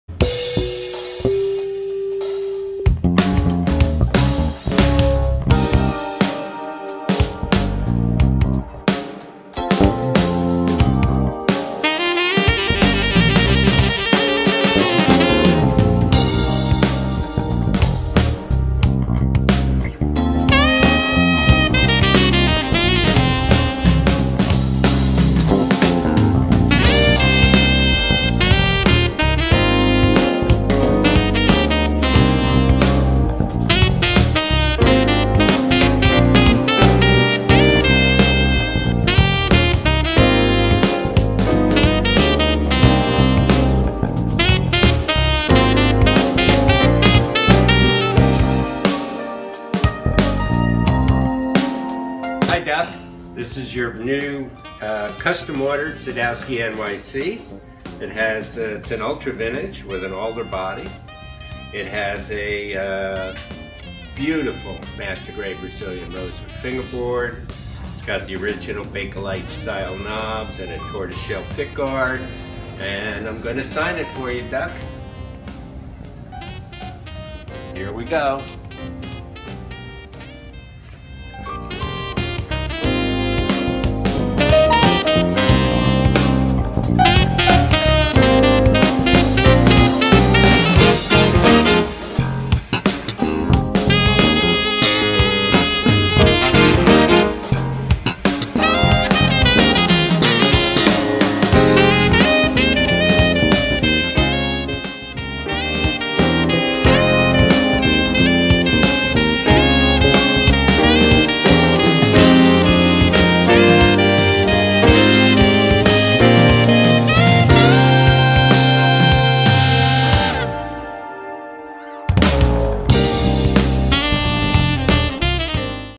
커스텀 베이스 완성 인터뷰 -Sadowsky Guitars Tour DuckBass
덕베이스에서 커스텀 오더 진행한 베이스.